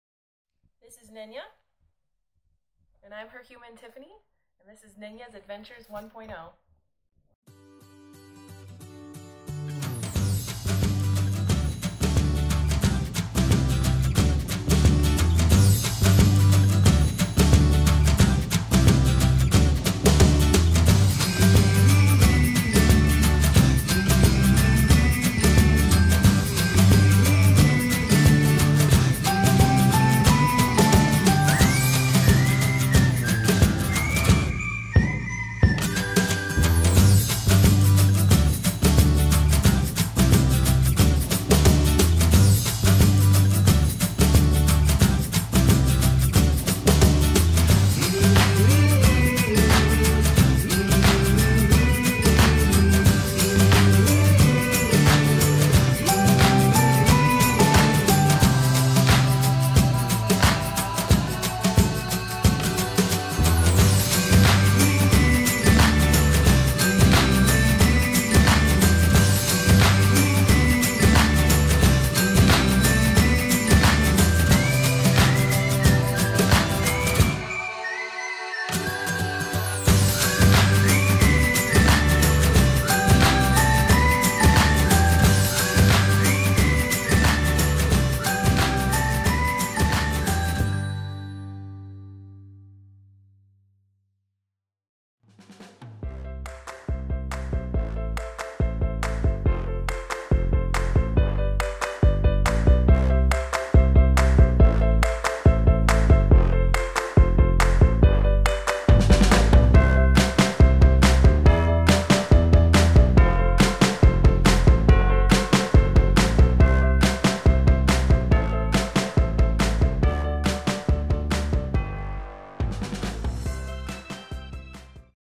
Siberian Husky: Hiking Devil’s Bridge in Sedona | Nenya’s Adventures 1.0
Hi guys! Today, I'm sharing with you the adventure Nenya, my Siberian Husky, and I had while Hiking Devil’s Bridge in Sedona, AZ

Nenya very much enjoys exploring new areas, seeing new sights, smelling new smells, and getting outside for exercise! And she likes to bring her humans with her.
 Quick FYI: Siberian Huskies are great companions for hiking since they essentially never get tired. I’m not even exaggerating. She can go on 5 hour hikes up mountains and act like it’s nothing. -.-
 Since we recently added Gunner to the family, we will be including him in our upcoming adventures as well!  In January, we hiked Devil’s Bridge.  Devil’s Bridge is a hike located in Sedona, AZ. It is one of the most popular hikes in the area. You’ll probably see around 50 cars parked around the parking area for this hike on the weekends. The parking lot fills up fast!

It is the largest sandstone arch in all of Sedona. So it’s not technically a bridge and despite the ominous sounding name, the place is gorgeous! But falling would be deadly …
We didn’t cross the top of the bridge. Even though Nenya is fearless, I am not and I didn’t want to take any chances, especially with her.

I will admit that it is not the easiest hiking trail for bringing your dog along, but it’s manageable. It just gets very steep at the end where you’re almost rock climbing, and that can make things a little tricky. Nenya doesn’t have a problem climbing them. It’s me worried about being accidently pulled over by her while climbing and faceplanting into boulders… Ouch. But we’ve managed to do it twice just very carefully and I have to admit, it is one of the prettiest trails in Arizona. I can see why it’s so popular.